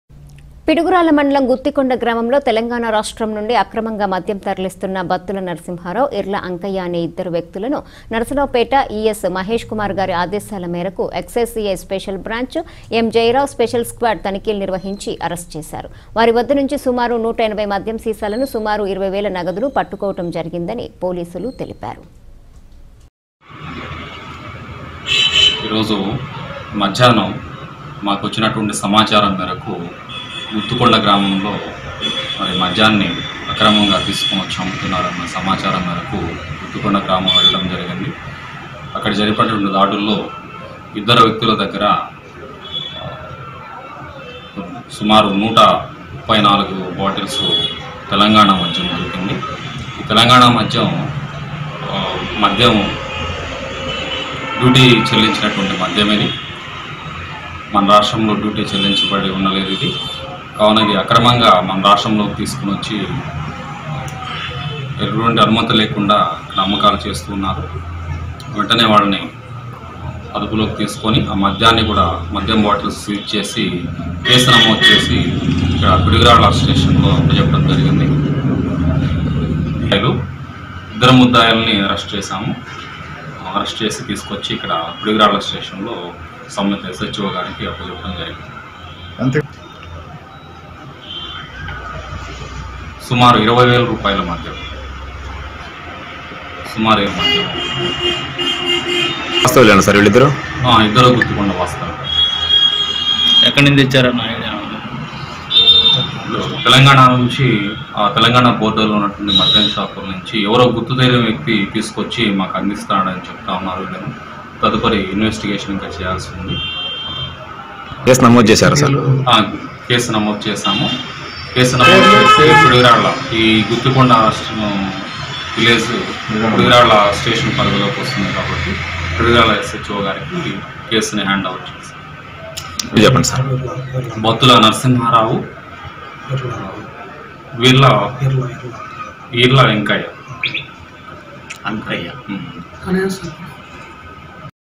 పిడుగురాళ్ళమండలం గుత్తికొండ గ్రామంలో తెలంగాణరాష్ట్రంనుండి అక్రమంగామద్యంతరలిస్తున్న బత్తులనరసింహారావు
పిడుగురాళ్ళ మండలం  గుత్తికొండ గ్రామంలో తెలంగాణ రాష్ట్రం నుండి అక్రమంగా మద్యం తరలిస్తున్న బత్తుల నరసింహారావు , ఇర్ల అంకయ్య అనే ఇద్ద‌రు వ్య‌క్తుల‌ను నరసరావుపేట ,ES, మహేష్ కుమార్ గారి ఆదేశాల మేరకు ఎక్సైజ్ సిఐ స్పెషల్ బ్రాంచ్,  ఎం. జయ రావు స్పెషల్ స్క్వాడ్ త‌నిఖీలు నిర్వ‌హించి అరెస్టు చేశారు. వారి వ‌ద్ద నుంచి సుమారు 180 మ‌ద్యం సీసాల‌ను, సుమారు రూ.20 వేల న‌గ‌దును  పట్టుకోవడం జరిగిందని పోలీసులు తెలిపారు .